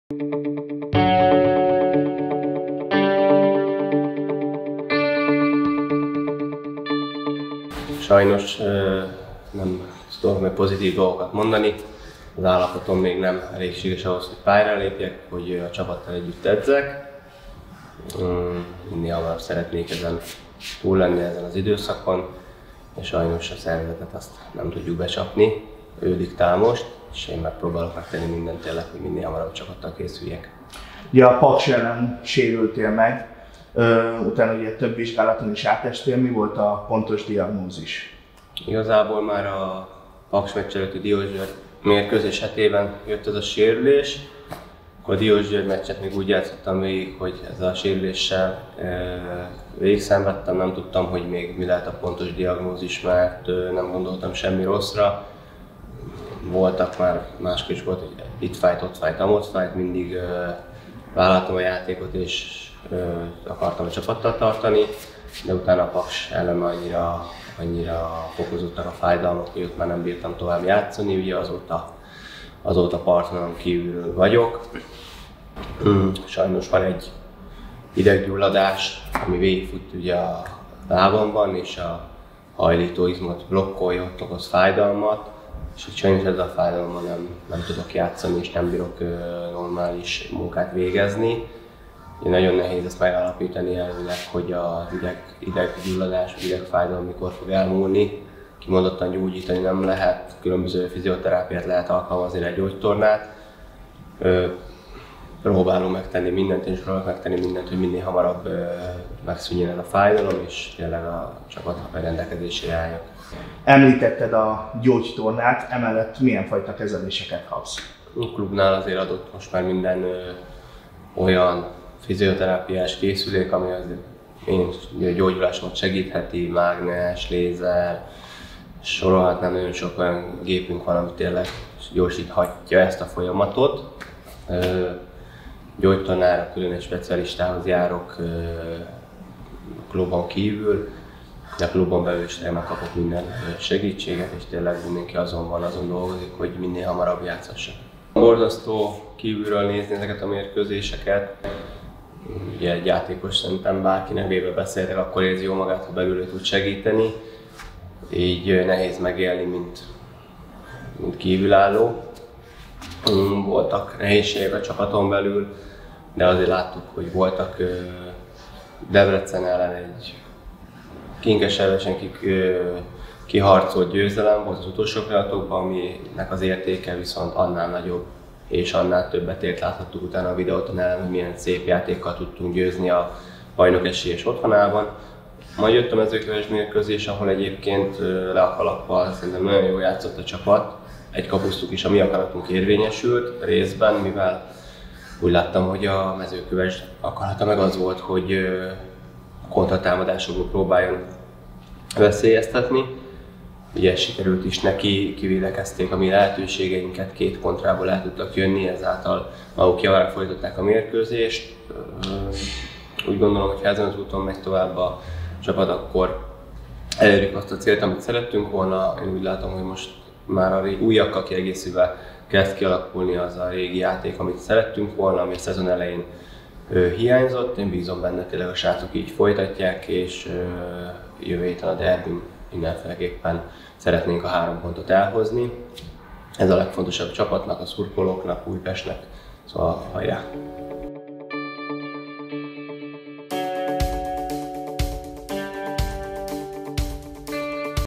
Hogy vagy, Lita?
Csapatunk kapitánya a Paksi FC elleni találkozón sérült meg, még augusztus közepén. Kíváncsiak voltunk arra, hogy hol tart most a felépülése!